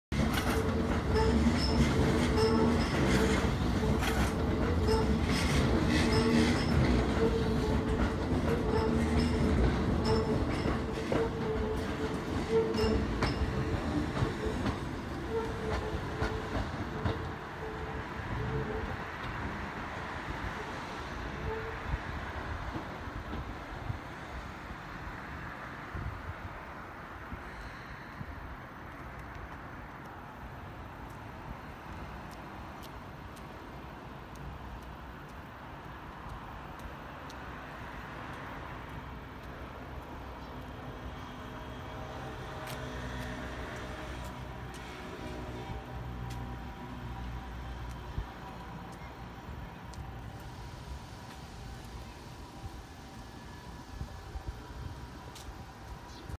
京急線4両編成13普通浦賀行き1000形449〜452編成ステンレス車両の電車が、金沢文庫駅方向から歩道橋の下を通過して車庫内を走行中！停車！
京急線金沢文庫駅手前の歩道橋の上から撮影！
金沢文庫駅方向から歩道橋の下を通過した、4両編成13普通浦賀行き1000形449〜452編成ステンレス車両の電車が、車庫内を走行！停車！
平成30年12月1日16時25分撮影！